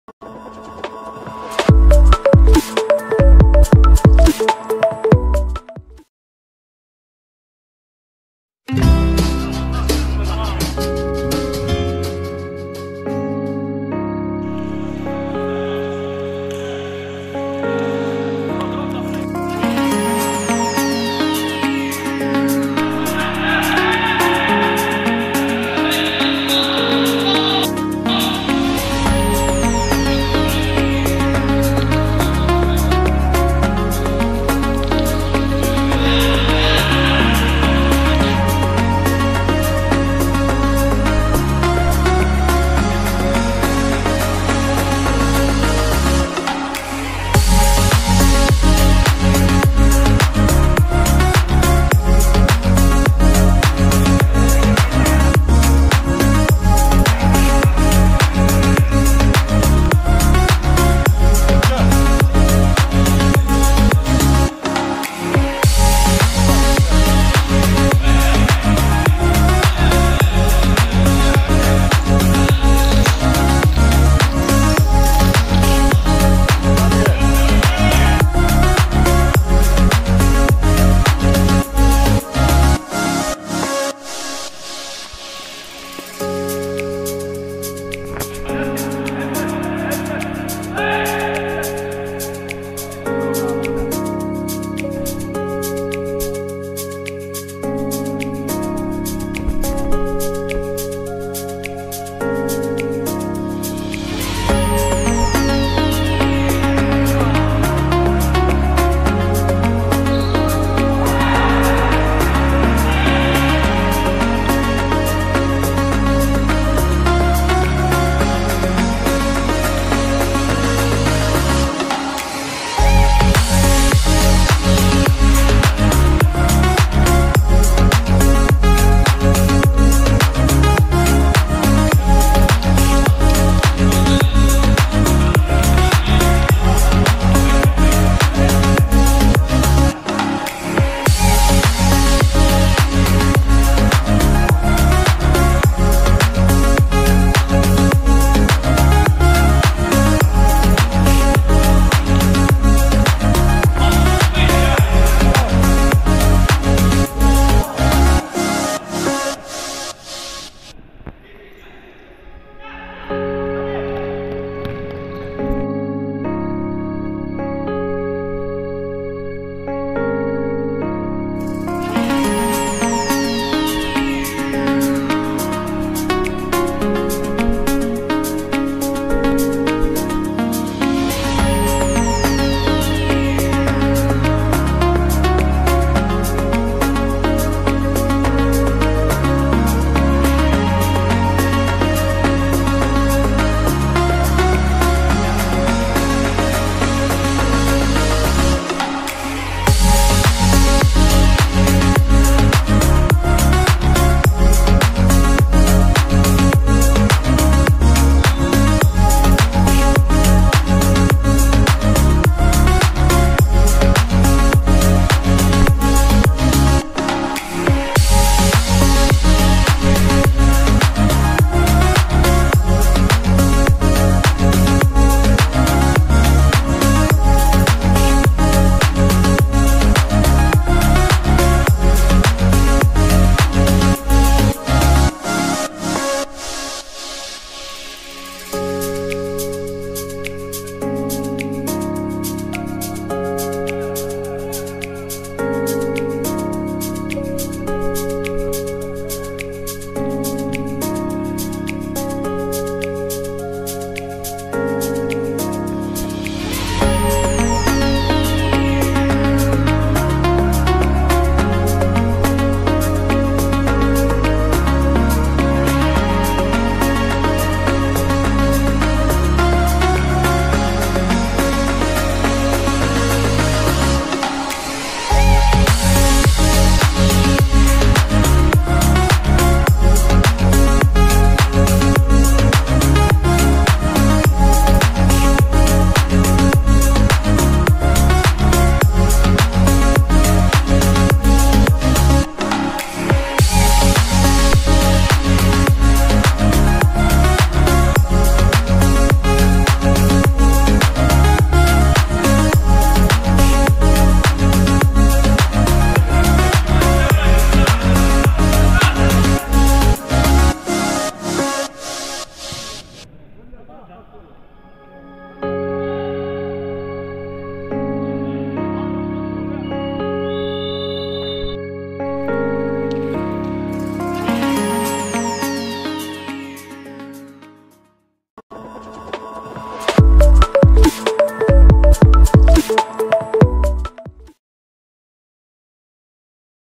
مقتطف من مباراة اصدقاء مرتيل و سطاد المغربي برسم الدورة 20 من دوري المغربي 2 فوتسال
مقتطف من مباراة اصدقاء مرتيل و سطاد المغربي  برسم الدورة 20 من دوري المغربي 2 فوتسال، بقاعة المغطاة ام كلثوم مرتيل انتهت لاصحاب الأرض بنتيجة 5-2.

الفايسبوك :

النسخة الفرنسية

 النسخة العربية

عشاق الرياضة

 التويتر

الأنستغرام

البريد الإلكتروني